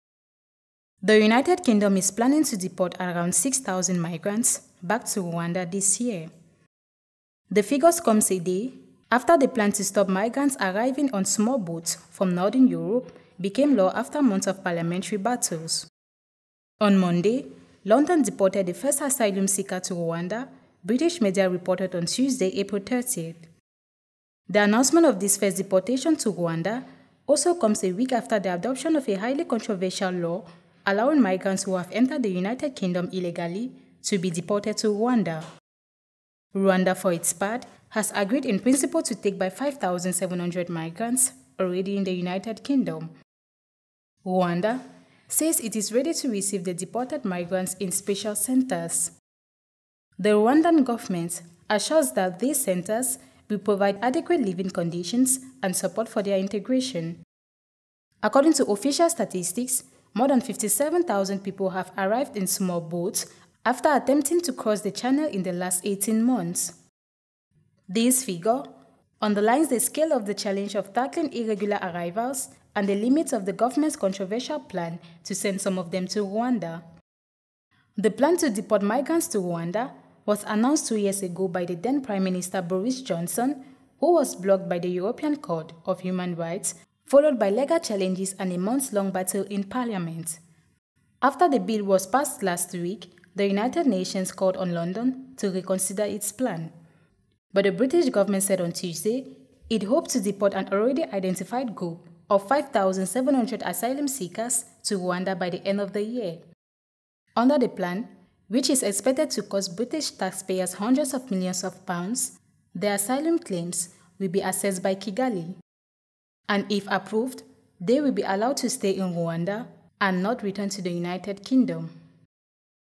UK TO DEPORT 6,000 MIGRANTS TO RWANDA IN COMING MONTHS
The UK will deport nearly 6,000 migrants to Rwanda this year, a senior minister said on Tuesday (30 April), as the government released new details of the controversial plan. The figures come days after the plan to deter migrants arriving on small boats from northern Europe became law after months of parliamentary wrangling.